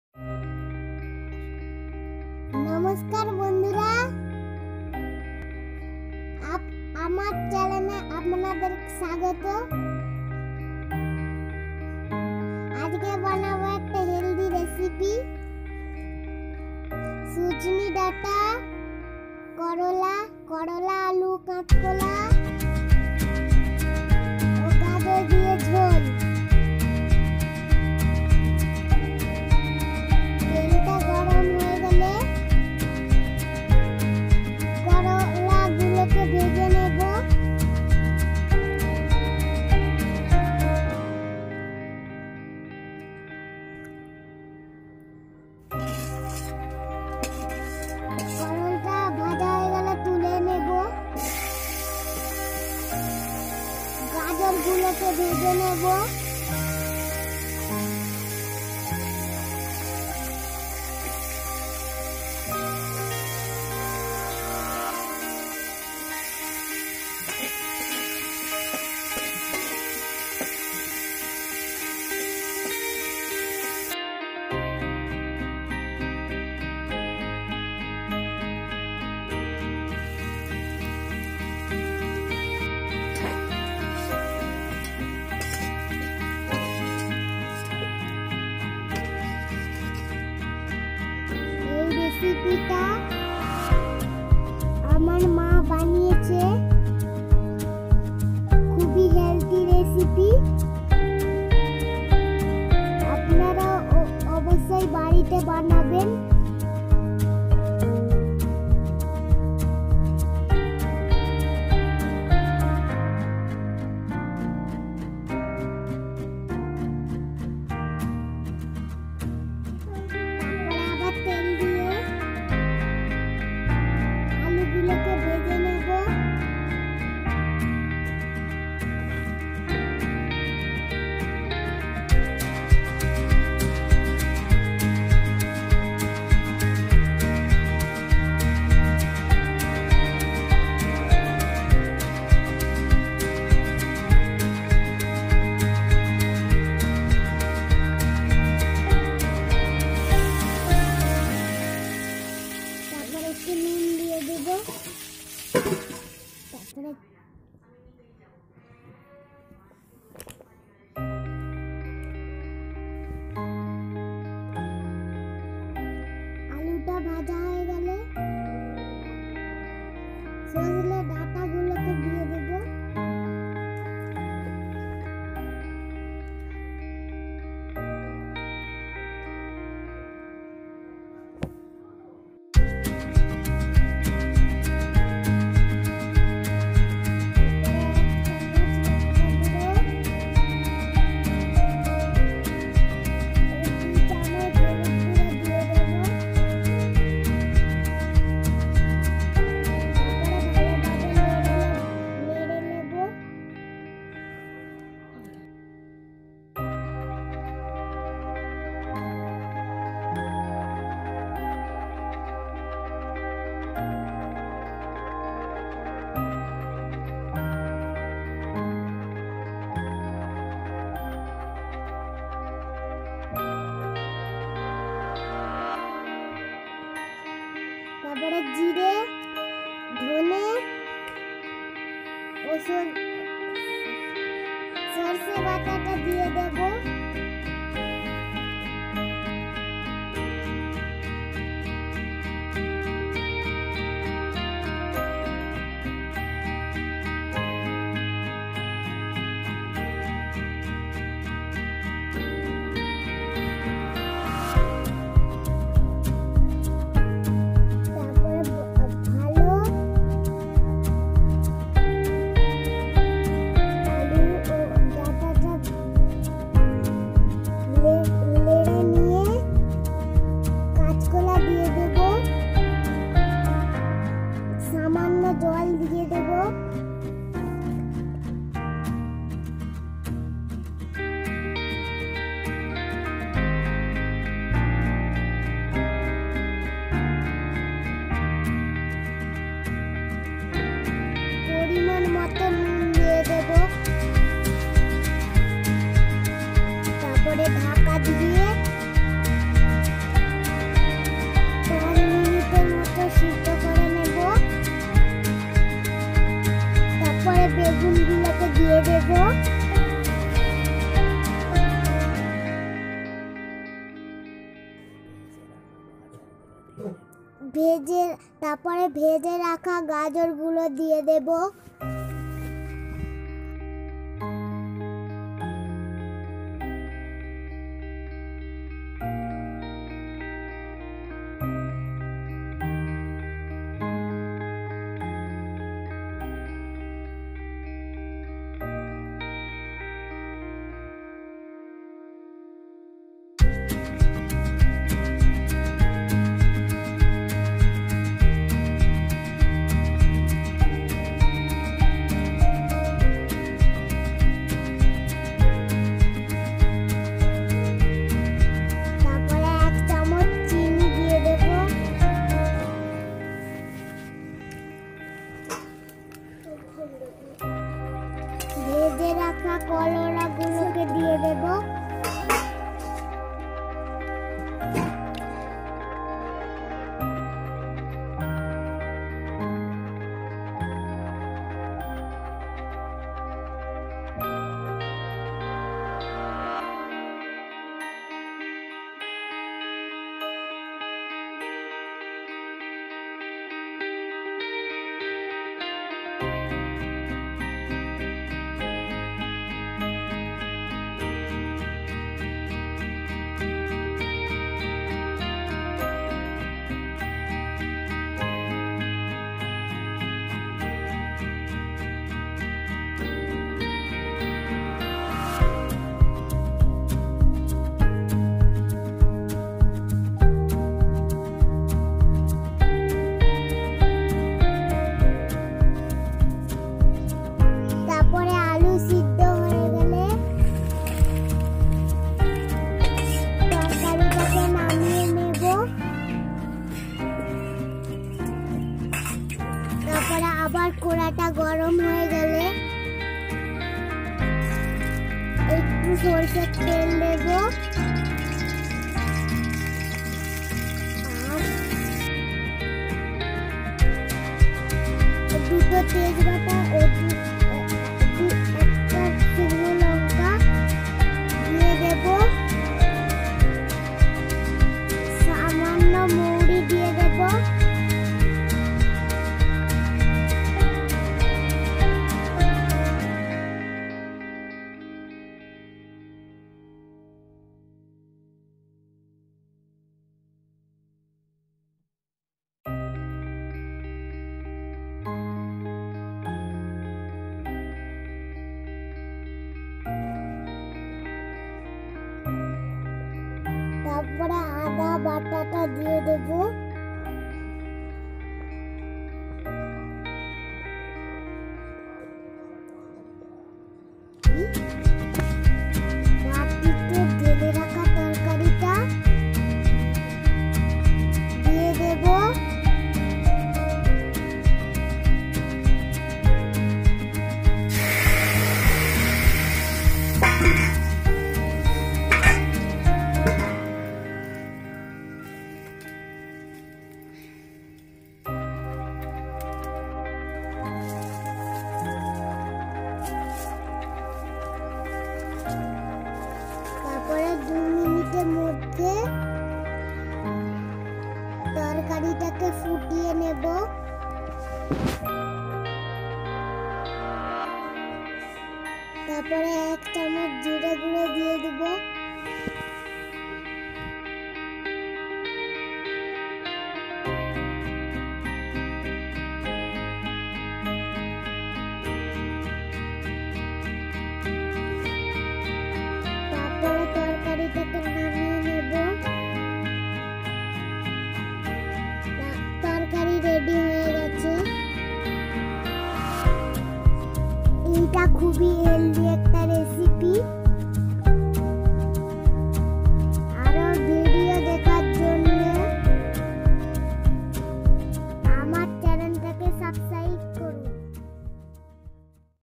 Sojne Data'r Panchmisali/ Drumsticks In A Light Vagetable Medley
welcome to my channel cooking with children 😀 today recipe sojne Data'r jhol niramish healthy village style recipe ingredent drumsticks potato plantain balsam pear carrot brinjal / masala- adrak paste cumin musterd seed paste fenel seeds coriander bay leaf Red mirchi  turmaric & mirchi powder salt & cumin powder